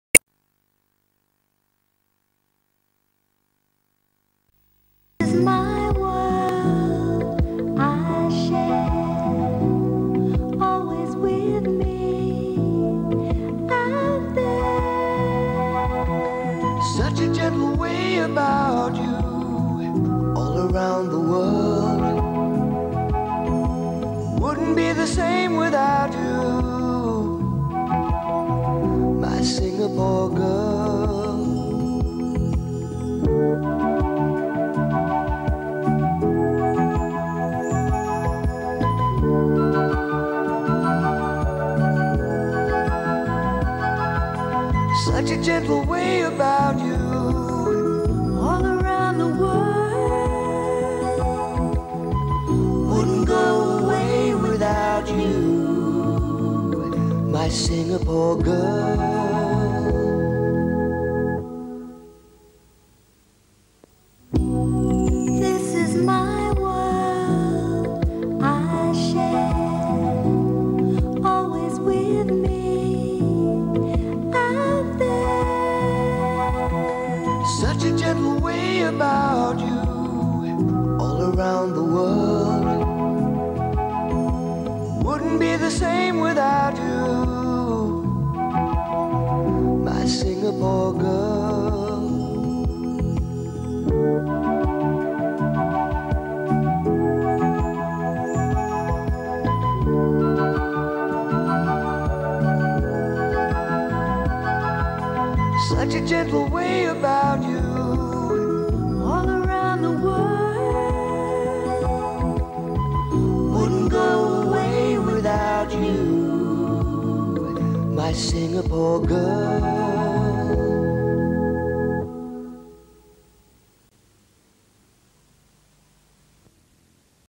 Dad'sSIASpot
Old video of Dad's SIA bit role in old SIA commercial